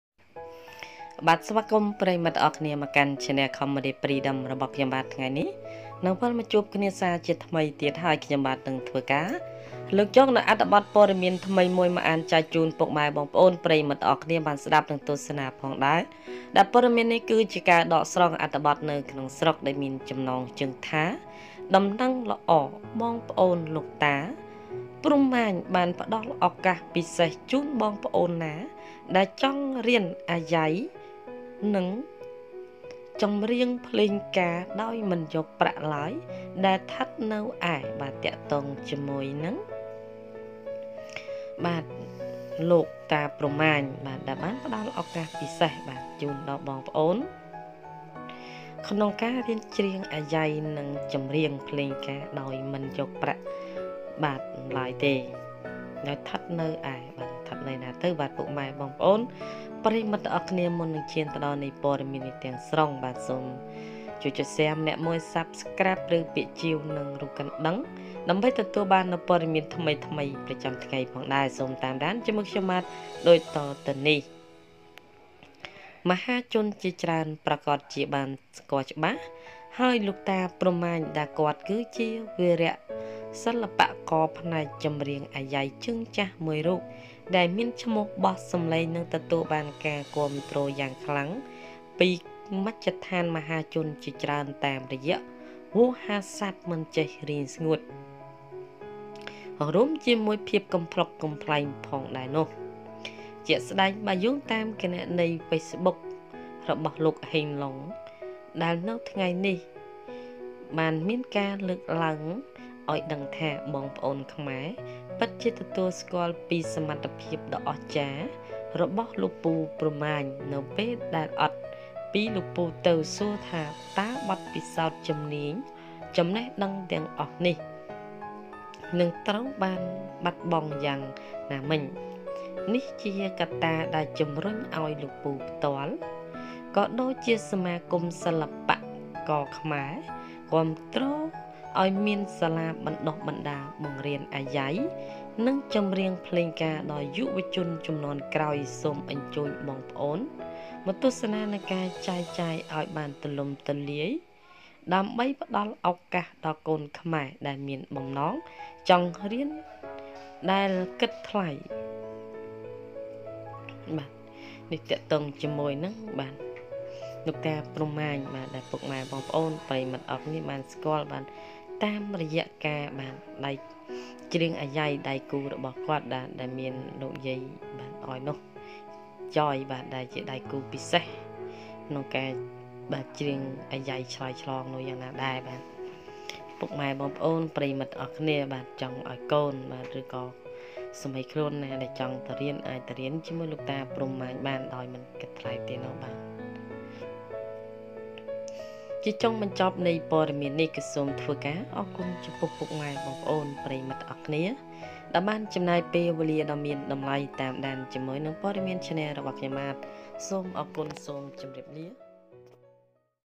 ដំណឹងល្អបងប្អូន! លោកតា​ ព្រហ្ម​ ម៉ាញ​ បានផ្តល់ឱកាសពិសេសជូនបងប្អូនណាដែលចង់រៀនអាយ៉ៃ និងចំរៀងភ្លេងការដោ
Good news, brothers!  Mr. Brahmaman gives a special opportunity to those who want to learn the art of music and music for free.

សូមស្វាគមន៍មកកាន់បណ្តាញព័ត៌មានសូមកុំភ្លេចចុះឈ្មោះជាវប៉ុស្តិ៍របស់ខ្ញុំសូមអរគុណ!

Cambodia Freedom

សូមអរគុណយ៉ាងជ្រាវជ្រៅវ
Thank You So much !